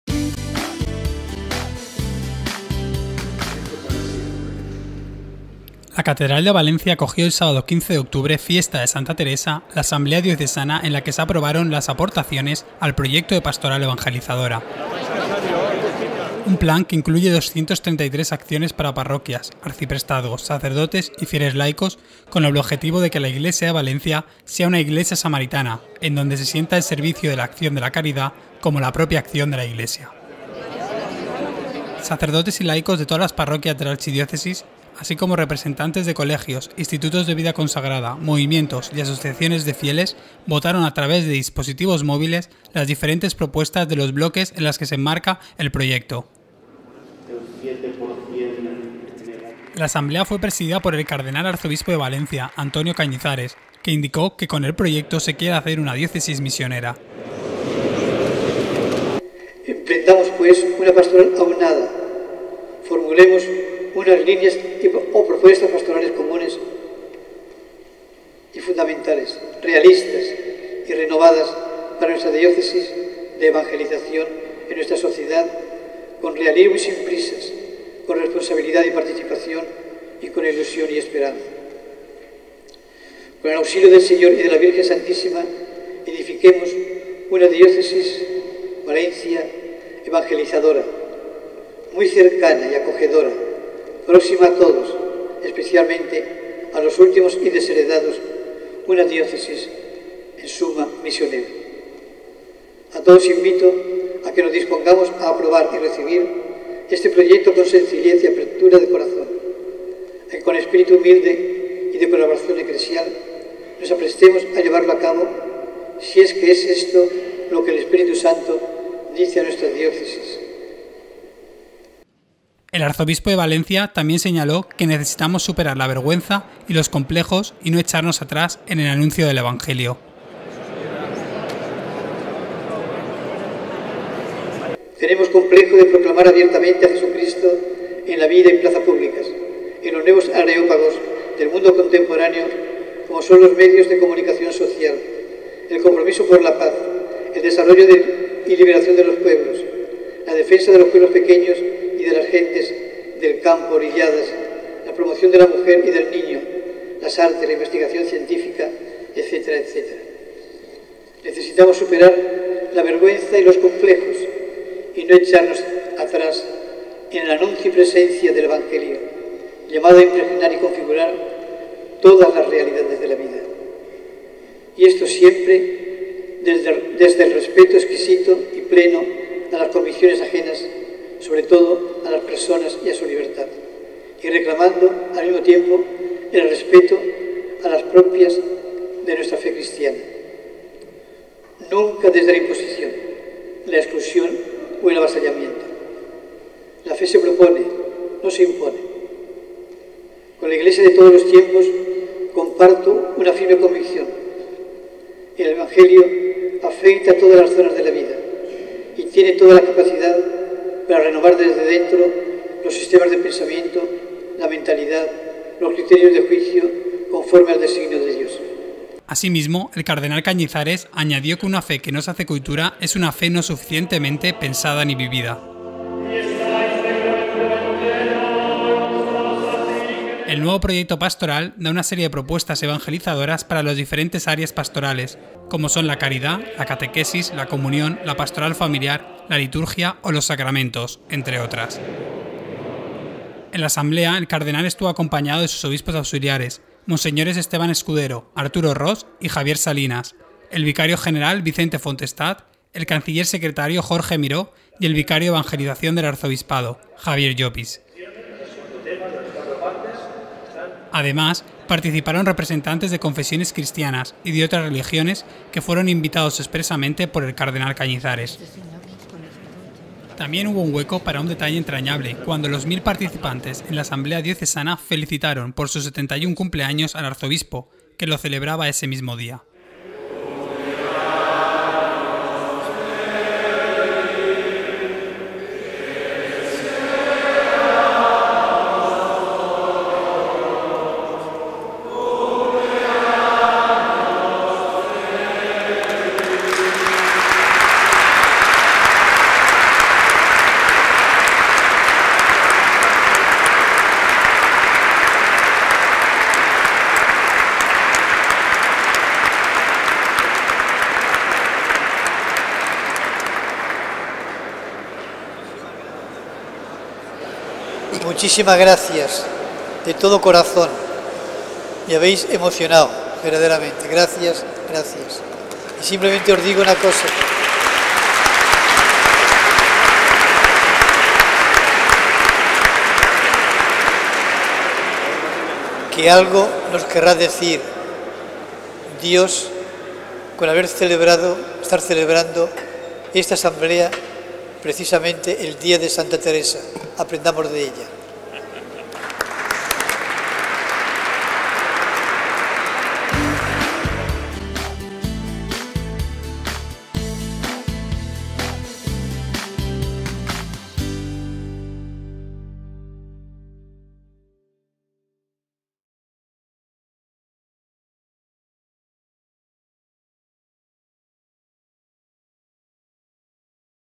Asamblea Diocesana en la Catedral de Valencia
Representantes de todas las parroquias aprueban en la Seo las propuestas para el nuevo Proyecto de Pastoral Evangelizadora.